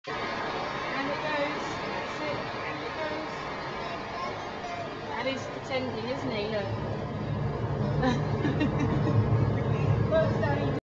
The drowning man
Me freaking my son out by pretending to drown behind a bubble window at the Natural History Museum. Recorded on January 24, 2009 using a Flip Video camcorder.